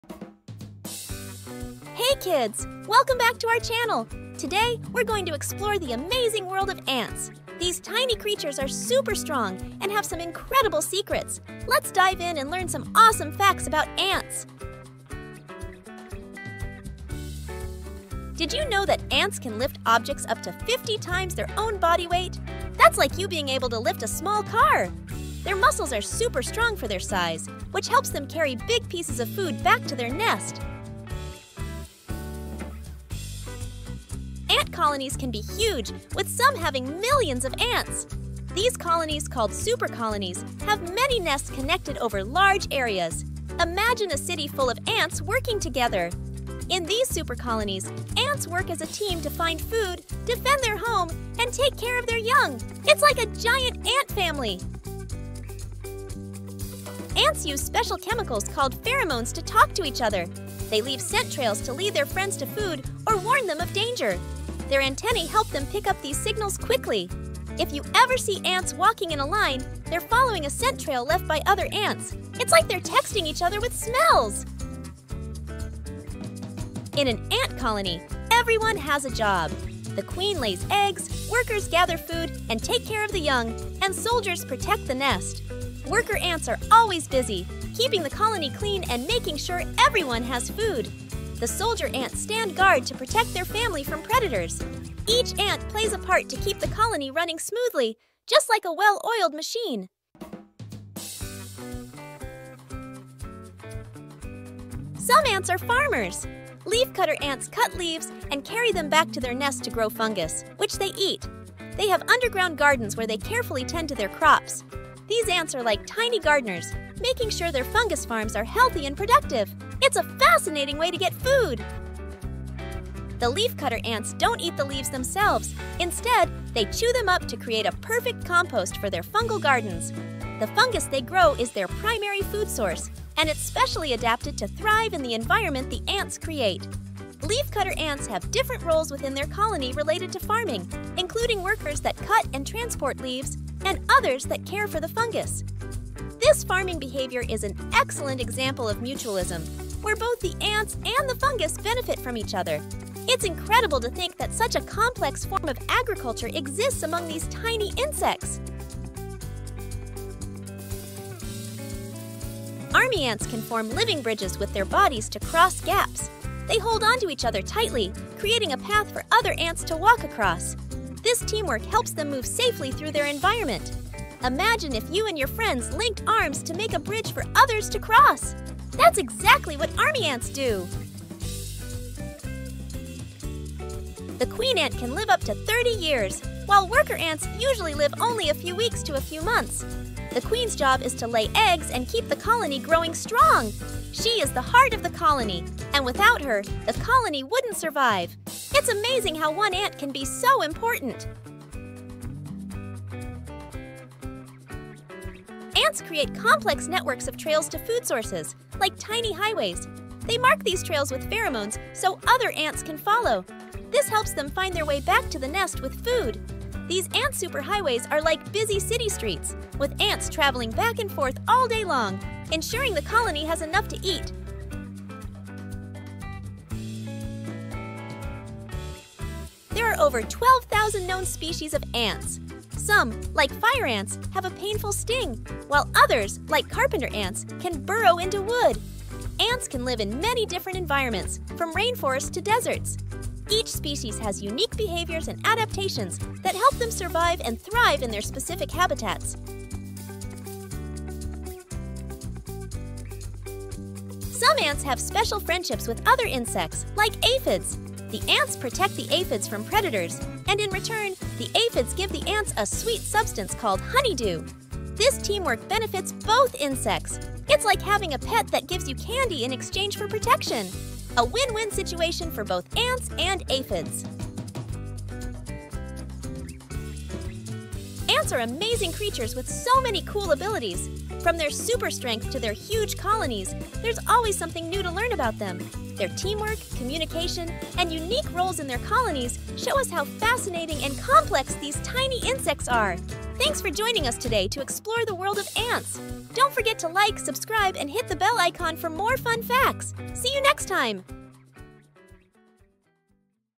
10 Fascinating Facts About Ants | Surprising Truths About One of Nature’s Hardest Workers
Welcome to the channel! Today, we’re venturing into the extraordinary world of ants. These tiny insects might be small, but they are among the most organized, powerful, and surprisingly intelligent creatures on Earth.

In this video, we’ll uncover 10 fascinating facts about ants that reveal their strength, communication, social structure, and even agricultural habits. From building supercolonies to forming living bridges and farming fungus, ants display some of the most advanced behaviors in the insect world.

What you’ll learn:
• Ants can lift up to 50 times their own body weight
• Some ant supercolonies stretch across entire continents
• How ants communicate through chemicals called pheromones
• The complex roles of queens, workers, and soldiers
• Leafcutter ants and their underground farming
• How ants form living bridges to solve problems
• The long reign of queen ants
• Intricate ant trail systems—nature’s superhighways
• The incredible diversity of over 12,000 ant species
• How ants form symbiotic partnerships with other species

Whether you’re fascinated by nature, insects, or just curious about how the smallest creatures can do such big things, this video is packed with insights.